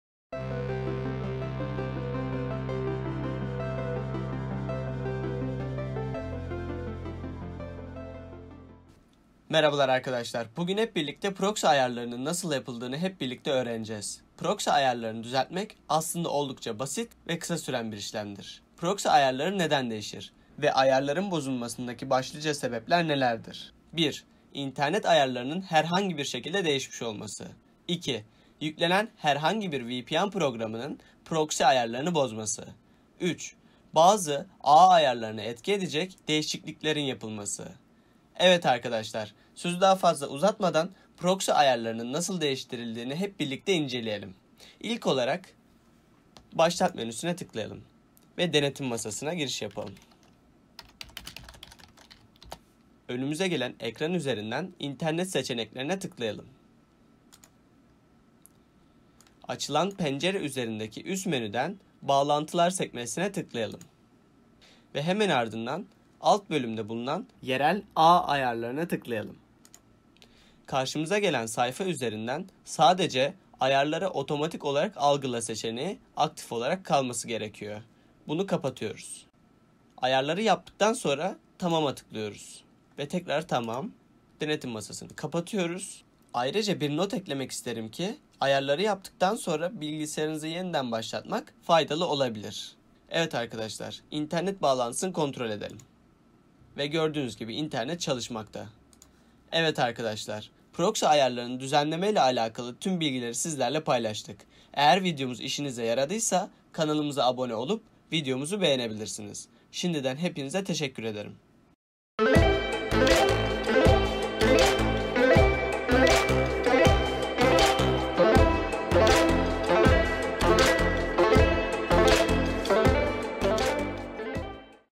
Proxy Ayarı Nasıl Yapılır? [Kesin Çözüm]
Merhabalar arkadaşlar bugün hep birlikte proxy ayarlarının nasıl yapılacağını hep birlikte öğreneceğiz.

Proxy ayarlarını düzenlemek aslında oldukça basit ve kısa süren bir işlemdir.

Proxy ayarları neden değişir? ve Ayarların bozulmasındaki başlıca sebepler nelerdir?

1. İnternet ayarlarının her hangi bir şekilde değişmiş olması.
2. Yüklenen her hangi bir vpn programının, proxy ayalarını bozması.
3. Bazı, ağ ayarlarına etki edecek, değişikliklerin yapılması.

Evet arkadaşlar sözü daha fazla uzatmadan proxy ayarlarının nasıl düzeltildiğini hep birlikte inceleyelim.

İlk olarak;

Başlat menüsüne "Denetim Masası" a giriş yapıyoruz.

ve hemen ardından "İnternet Seçenekleri" sekmesine tıklıyoruz.

Açılan sekme üzerinden "Bağlantılar" bölümüne tıklıyoruz.

Bağlantılar üzerindeki alt bölümdeki "Yerel Ağ Ayarları" Bölümüne tıklıyoruz.

Açılan sayfada sadece "Ayarları otomatik olarak algıla" seçeneği aktif kalacak şekilde ayarlayıp. Tamam tuşuna basıyoruz.

Tekrar Tamam diyoruz.

Ayrıca bir not eklemek isterim ki; Bilgisayarımızı yeniden başlatmakta faydalı olabilir.